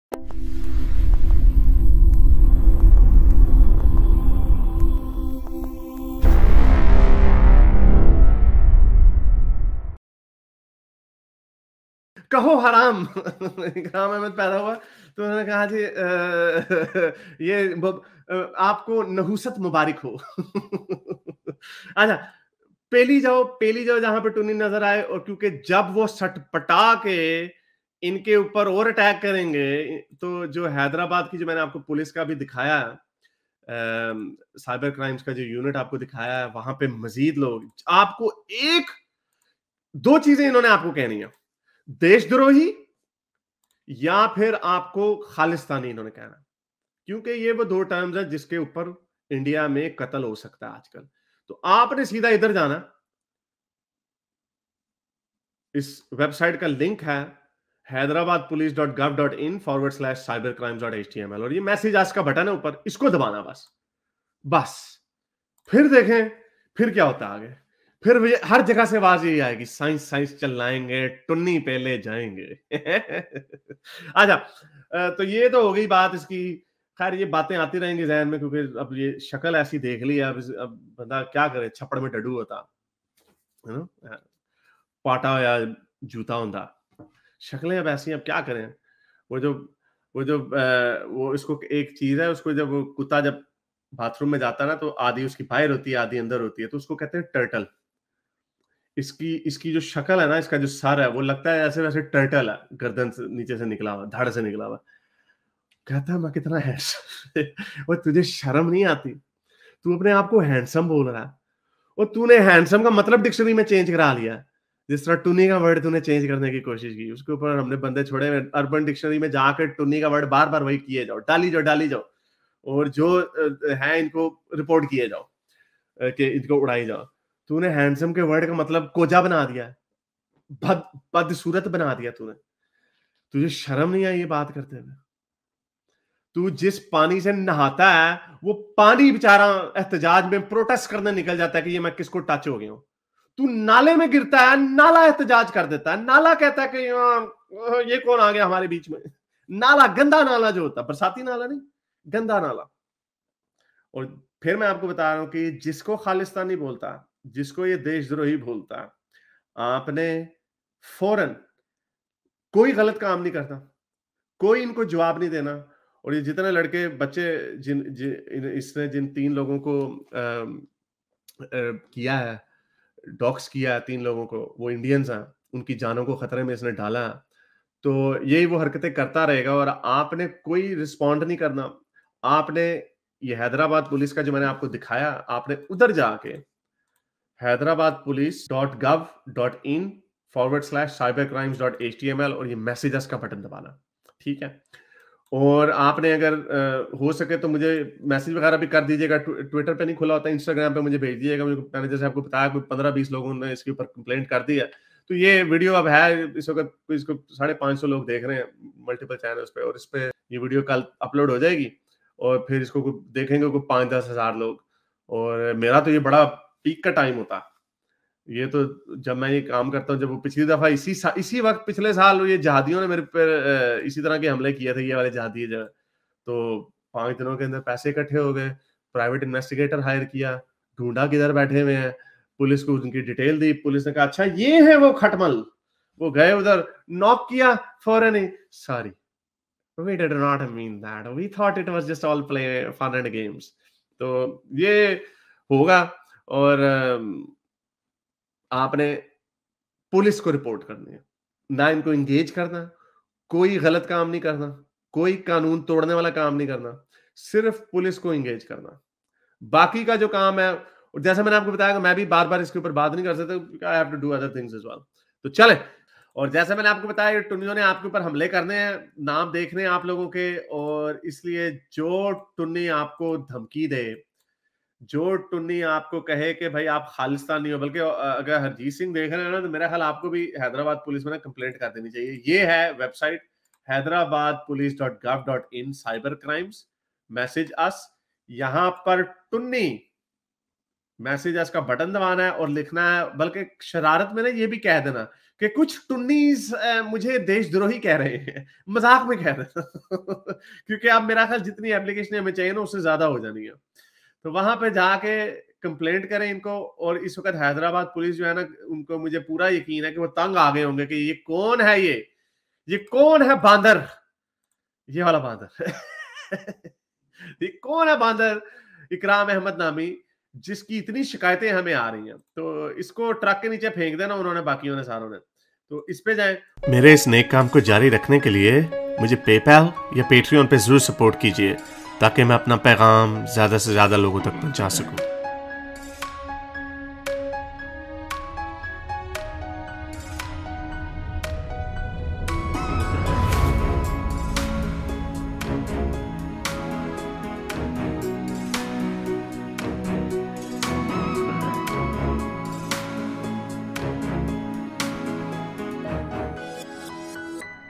Agar Koi Tunni Aapko Desh-Darohi Bolay to Hyderabad Police May Report Karain
Agar yeh tunni aapko desh-darohi ya Khalistunni bolay to issay Hyderabad Police may report karain